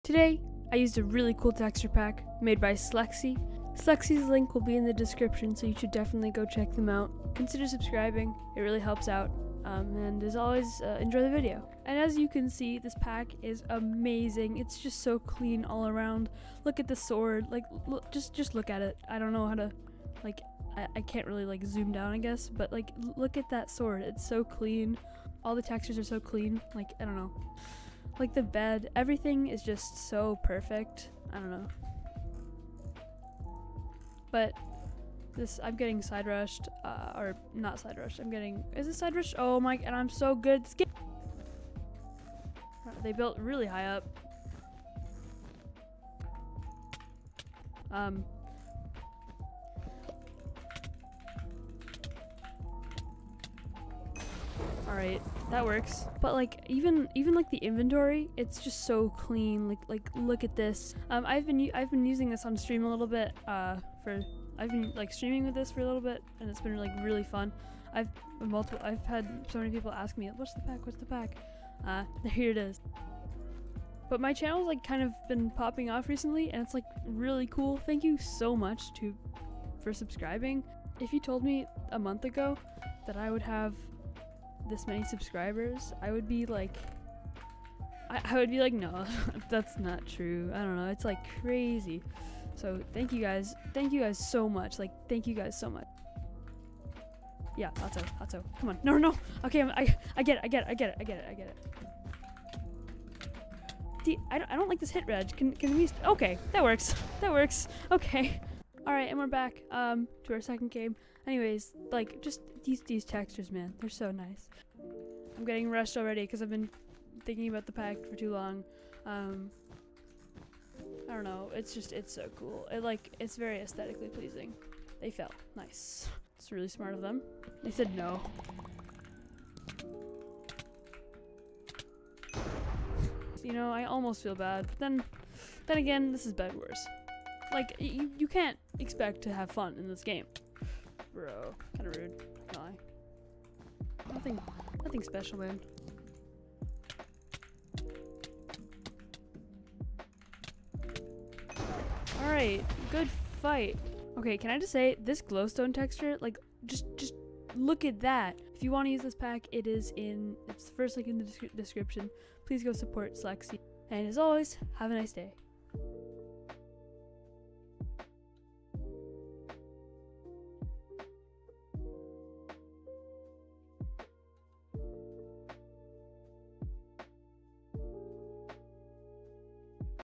Best 32x texture pack | Slexi Pepsi 32x (1.8.9)
if anyone has any problems with this video here's my discord and we can talk: Adafish#5420

This fps boosting pack is SO AMAZING!
I really hope you like it :) You all should def download.